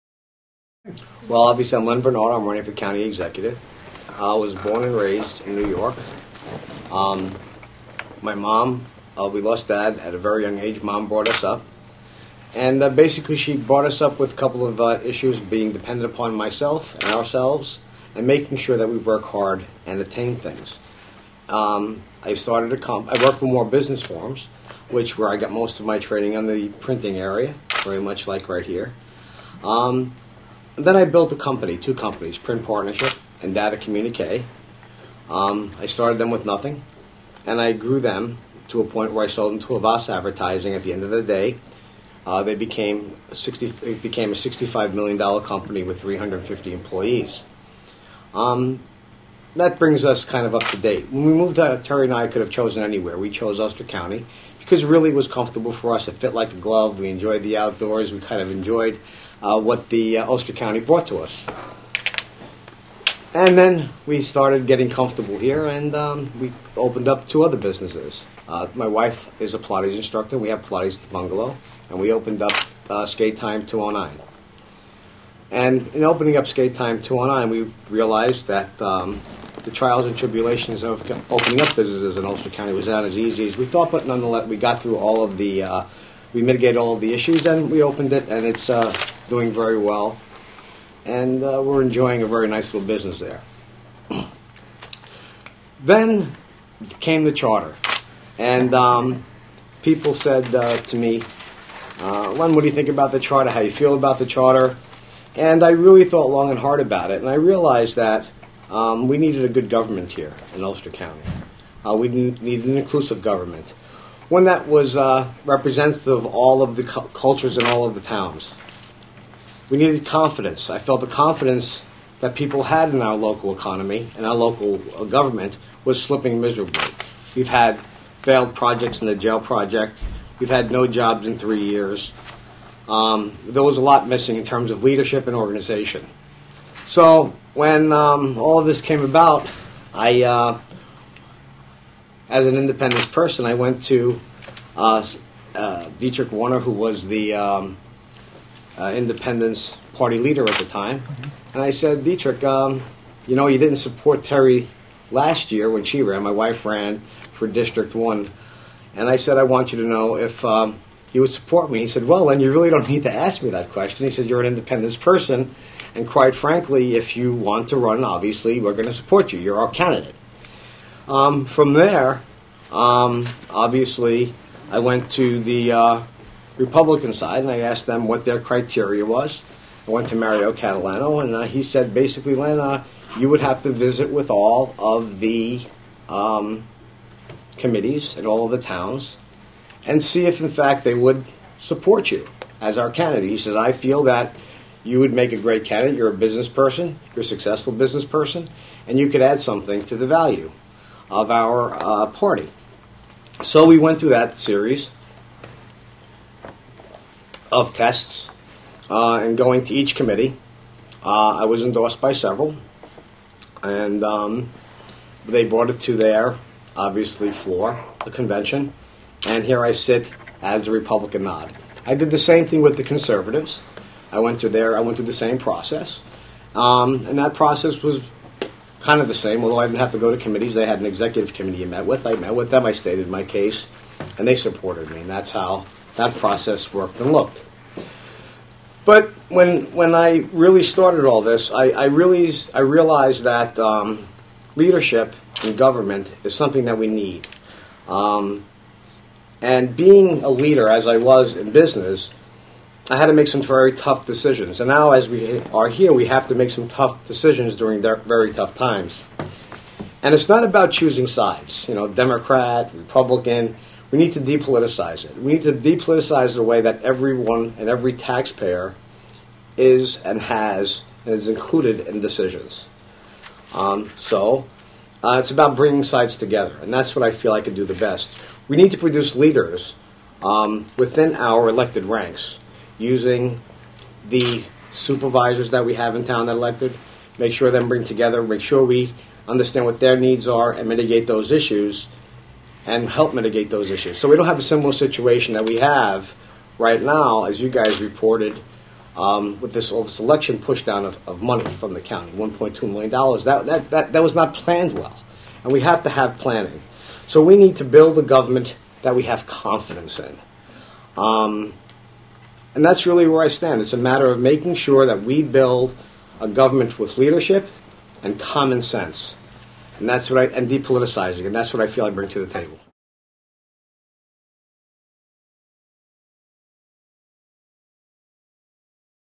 Ulster County Executive Race
Business owner Len Bernardo: R, I, C  is running against Ulster County Administrator Michael Hein: D, WF in the Ulster County Executive Race.